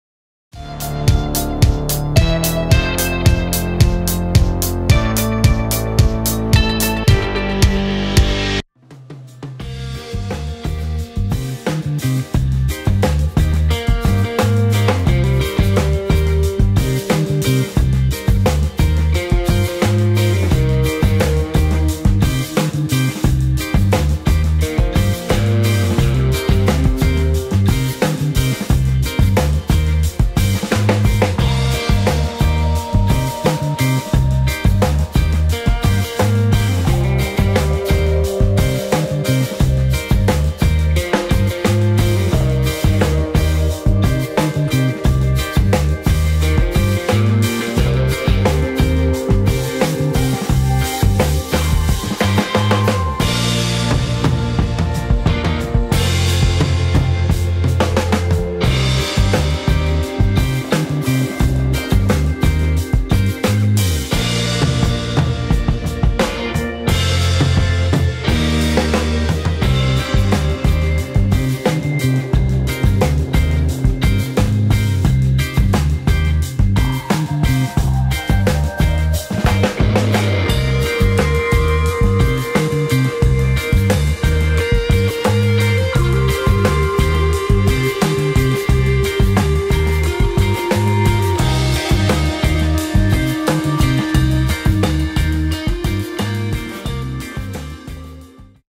Voeren zeewater vissen
Het voeren van de zeewater vissen is een leuke bezigheid. Het zien hoe je vissen toehappen op alles wat je in het aquarium aan vissenvoer gooit. De vissen en lagere dieren in het zeeaquarium lusten het graag. De anemoon wordt dankzij de Nemo's van het voer voorzien. De mossel ligt er stilletjes bij. Alles te zien in dit korte zeeaquarium filmpje.